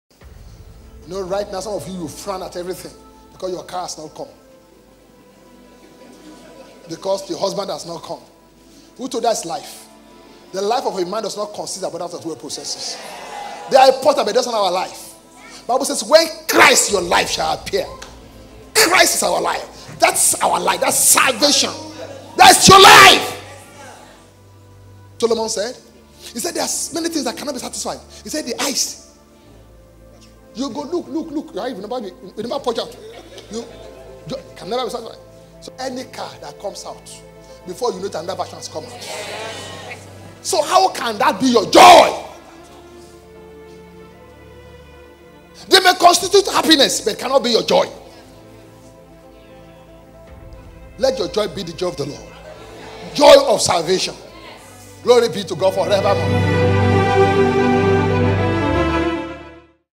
Exhortation - PASTOR TUNDE AYENI (GRACEVILLE CHRISTIAN CENTRE)
Ministering Pastor Tunde Ayeni
Pastor Tunde Ayeni is the Senior Pastor  of Graceville Christian Centre Abuja.